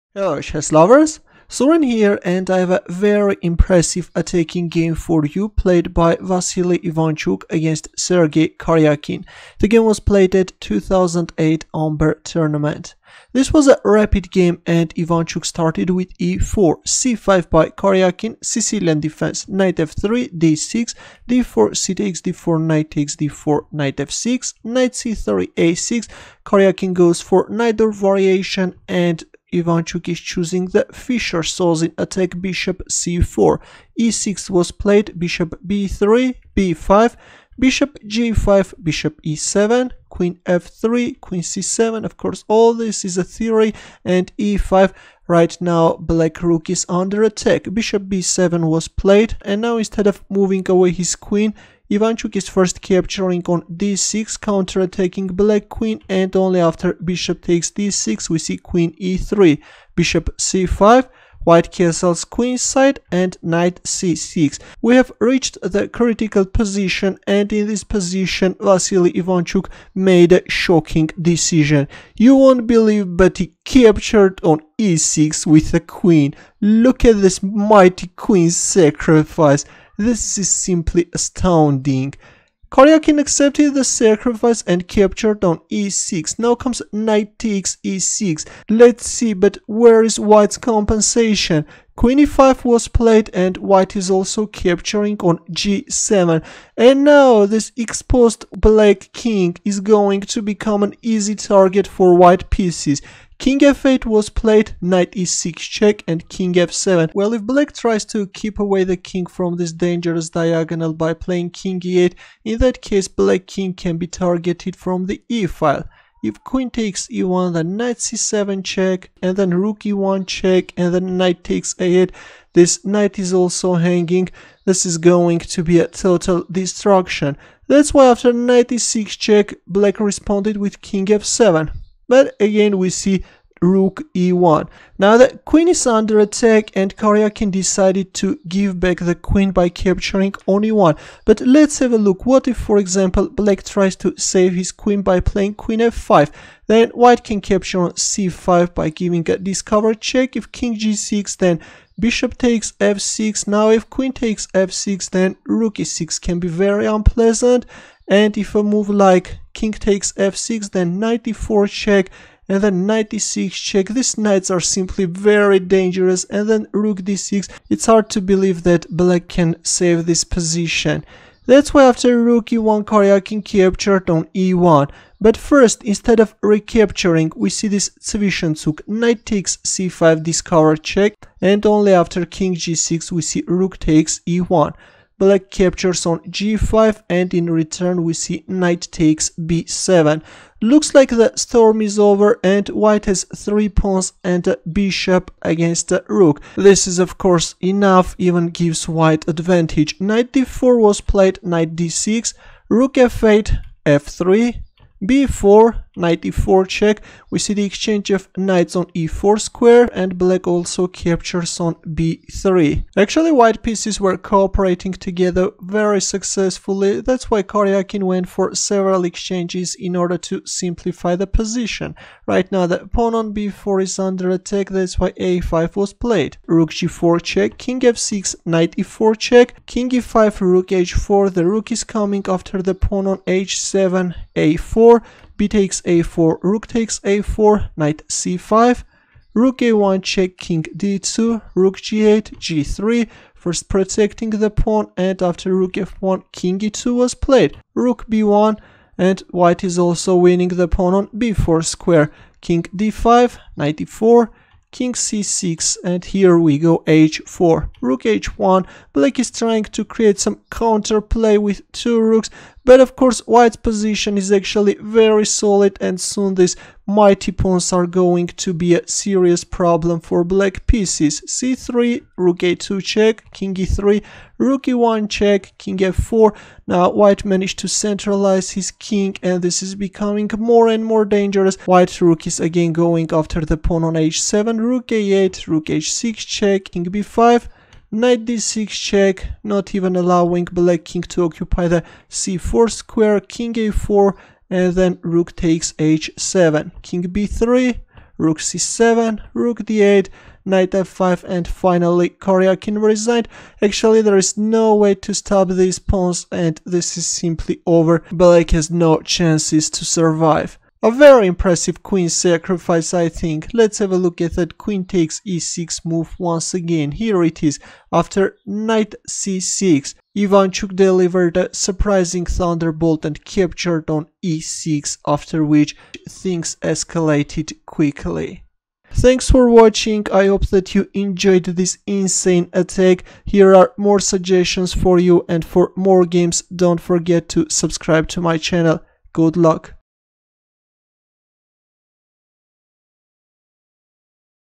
Absolutely Insane Queen Sacrifice By Vassily Ivanchuk
This is Ivanchuk vs Karjakin chess game analysis!
Vassily Ivanchuk (2751) vs Sergey Karjakin (2732)
Amber Tournament (Rapid) (2008), Nice FRA, rd 4, Mar-18
Sicilian Defense: Fischer-Sozin Attack. Flank Variation (B87)

1. e4 c5 2. Nf3 d6 3. d4 cxd4 4. Nxd4 Nf6 5. Nc3 a6 6. Bc4 e6 7. Bb3 b5 8. Bg5 Be7 9. Qf3 Qc7 10. e5 Bb7 11. exd6 Bxd6 12. Qe3 Bc5 13. O-O-O Nc6 14. Qxe6+ fxe6 15. Nxe6 Qe5 16. Nxg7+ Kf8 17. Ne6+ Kf7 18. Rhe1 Qxe1 19. Nxc5+ Kg6 20.Rxe1 Kxg5 21. Nxb7 Nd4 22. Nd6 Rhf8 23. f3 b4 24. Nce4+ Nxe4 25. Rxe4 Nxb3+ 26.axb3 a5 27. Rg4+ Kf6 28. Ne4+ Ke5 29. Rh4 a4 30. bxa4 Rxa4 31. Nc5 Ra1+ 32. Kd2 Rg8 33. g3 Rf1 34. Ke2 Rb1 35. Rxb4 Kd5 36. Ne4 Kc6 37. h4 Rh1 38. Rc4+ Kb6 39.b4 Rd8 40. Rc5 Ra8 41. c3 Ra2+ 42. Ke3 Re1+ 43. Kf4 Rf1 44. Rh5 Ra8 45. Rh6+ Kb5 46. Nd6+ Ka4 47. Rxh7 Kb3 48. Rc7 Rd8 49. Nf5
 1-0